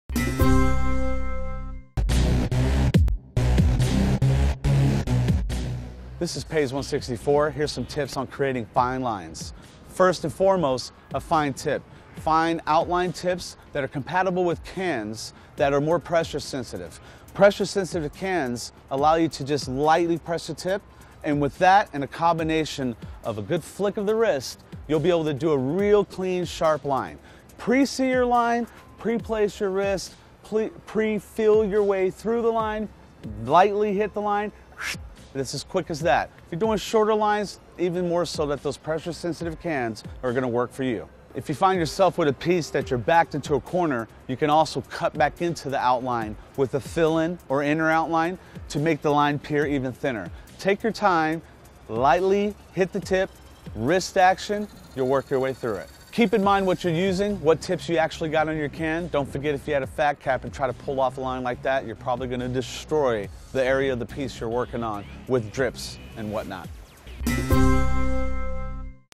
4 Tips on Creating Fine Lines | Graffiti Art
This is Paise [sp?] 164. Here's some tips on creating fine lines. First and foremost, a fine tip. Fine outline tips, that are compatible with  cans, that are more pressure sensitive. Pressure sensitive cans allow you to just lightly press the tip. And with that, and a combination of a good flick of the wrist, you'll be able to do a real clean, sharp line. Pre-see your line, pre-place your wrist, pre-feel your way through the line, lightly hit the line, and it's as quick as that.

If you're doing shorter lines, even more so that those pressure sensitive lines are going to work for you. If you find yourself with a piece that you're backed into a corner, you can also cut back into the outline with a fill-in, or inner outline, to make the line appear even thinner. Take your time, lightly hit the tip, wrist action, you'll work your way through it. Keep in mind what you're using, what tips you actually got on your can. Don't forget if you had a fat cap and try to pull off a line like that,  you're probably going to destroy the area of the piece you're working on, with drips and whatnot.